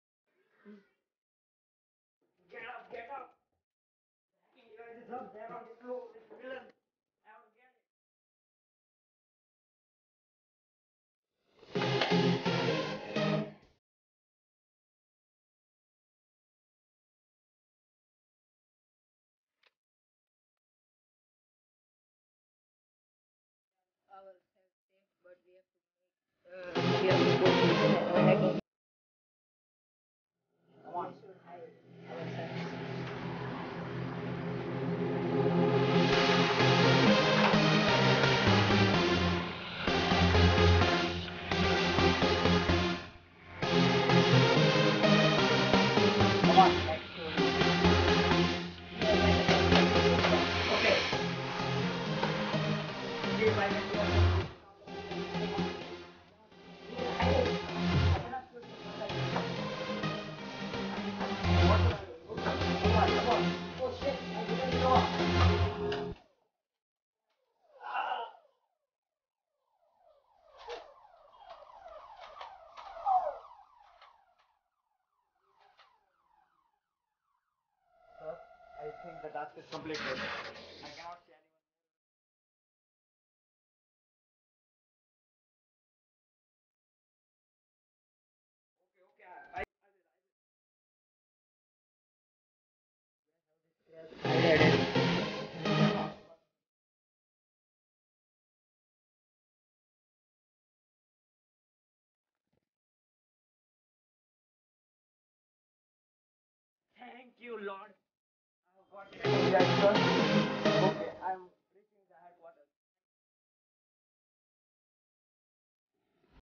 Let the world be saved...
Magma has to save the world by defeating 2 super powers - Johnny and Danny...What do you think is gonna happen to the world...Let's explore...